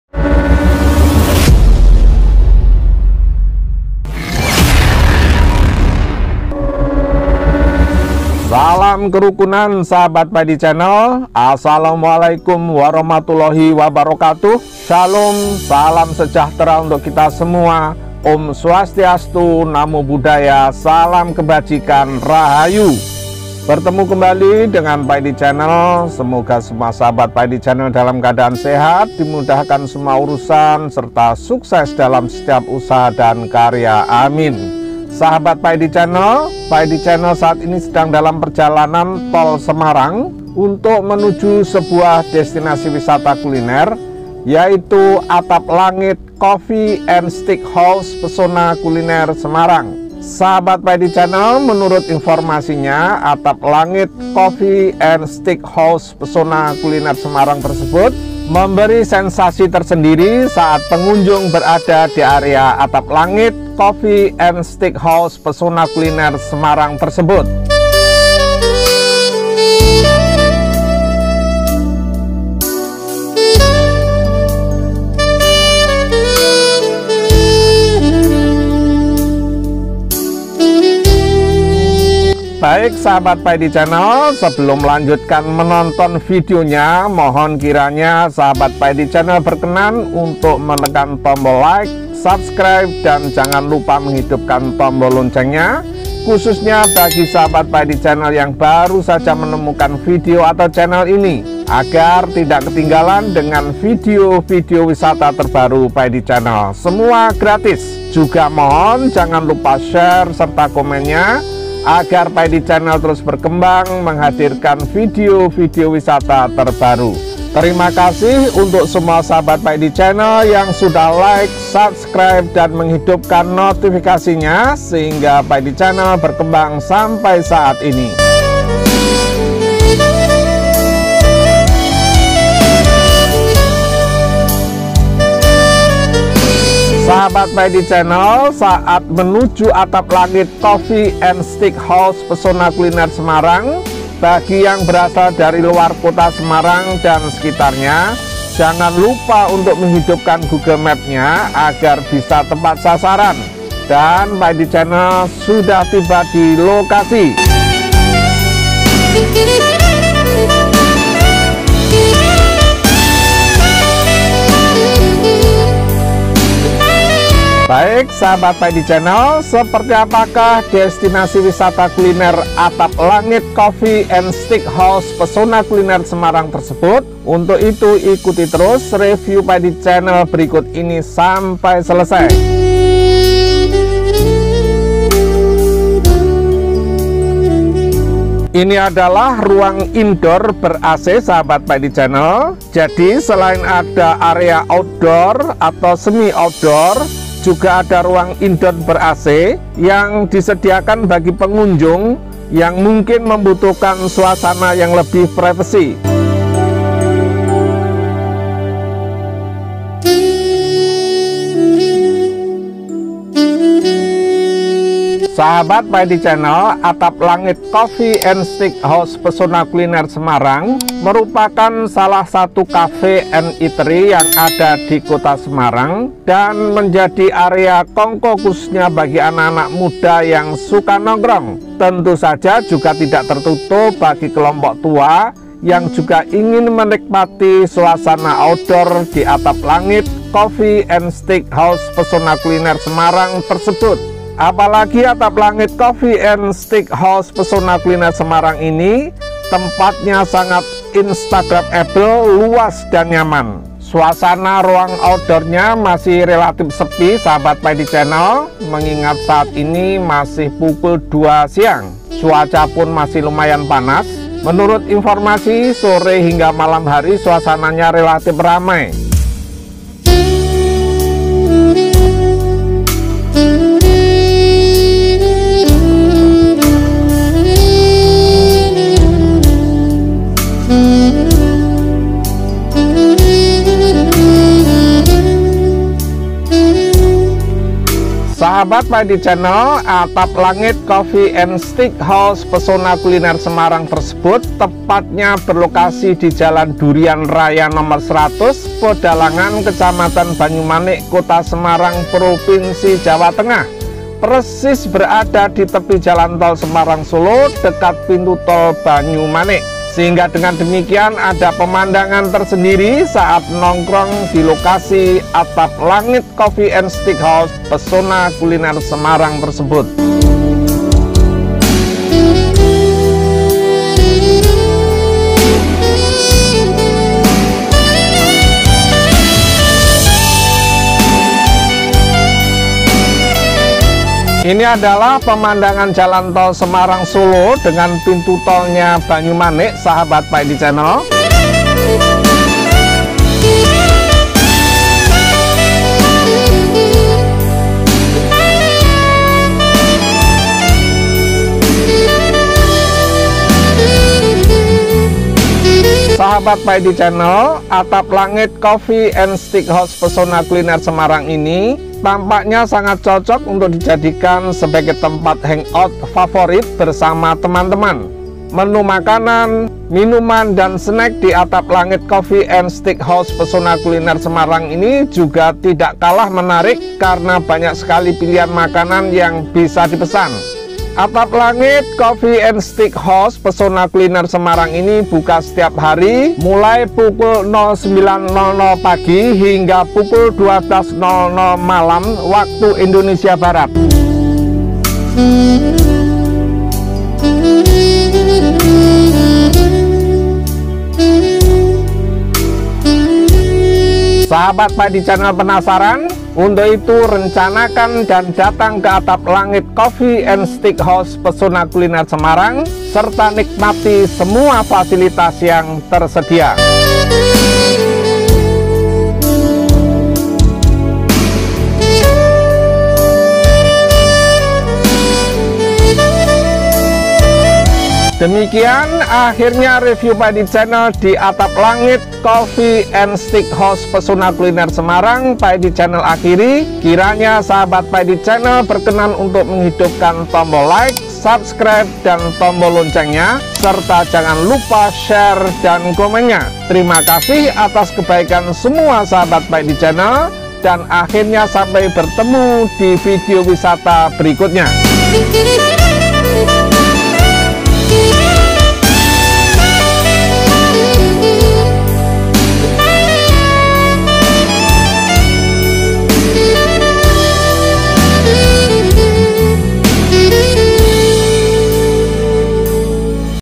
Atap Langit Coffee and Steak House Pesona Kuliner Semarang ~ Pa'Edi Channel
Pa'Edi Channel
Atap Langit Coffee and Steak House Pesona Kuliner Semarang merupakan salah Cafe and Eatery  ada di kota Semarang.
Atap Langit Coffee and Steak House Pesona Kuliner Semarang tsb menjadi area kongkow khususnya bagi anak muda suka nongkrong.
Apalagi Atap Langit Coffee and Steak House Pesona Kuliner Semarang ini
tempatnya sangat instagramable, luas dan nyaman.
Atap Langit Coffee and Steak House Pesona Kuliner Semarang tsb tepatnya berlokasi di Jl. Durian Raya No. 100, Pedalangan, Kec. Banyumanik, Kota Semarang, Jawa Tengah. Atap Langit Coffee and Steak House Pesona Kuliner Semarang letaknya persis di tepi jalan tol Semarang Solo, sehingga ada pemandangan tersendiri saat nongkrong di lokasi Atap Langit Coffee and Steak House Pesona Kuliner Semarang tsb.
Atap Langit Coffee and Steak House Pesona Kuliner Semarang ini menjadi tempat nongkrong yang menarik karena area  yang sangat luas dan didesain untuk anak muda banget. Daya tarik utama dari Atap Langit Coffee and Steak House Pesona Kuliner Semarang
adalah tempatnya yang instagramable dan sangat asyik untuk dijadikan spot nongkrong bareng teman. Ada ruang out door terbuka luas, ada semi out door dan juga ruang indoor ber AC, juga ada lantai dua.
Area Atap Langit Coffee and Steak House Pesona Kuliner Semarang baik indoor dan outdoor yang sangat luas ditata sedemikian rupa sehingga terlihat sangat estetik. Area outdoor menjadi daya tarik utama dan favorit para pelanggan karena berupa area duduk yang sangat luas.
Di area outdoor ini pelanggan bisa memilih tempat duduk sesuai keinginannya berupa lesehan atau kursi yang semuanya nyaman.
Atap Langit Coffee and Steak House Pesona Kuliner Semarang ini cocok dijadikan sebagai tempat hangout favorit bersama teman. Menu makanan, minuman dan snack di Atap Langit Coffee and Steak House Pesona Kuliner Semarang ini juga tidak kalah menarik karena banyak sekali pilihan makanan yang bisa dipesan.
Atap Langit Coffee and Steak House Pesona Kuliner Semarang ini buka setiap hari pada jam 09.00 – 24.00 WIB. Selain melayani pesanan di tempat, dan ternyata Atap Langit Coffee and Steak House Pesona Kuliner Semarang juga melayani takeaway & delivery service.
Berbagai macam fasilitas jugs tersedia di Atap Langit Coffee and Steak House Pesona Kuliner Semarang tsb seperti :
Special good & Beverage, Indoor/outdoor toom, Wifi, photo spot, musola, toilet, area parkir luas, dsb.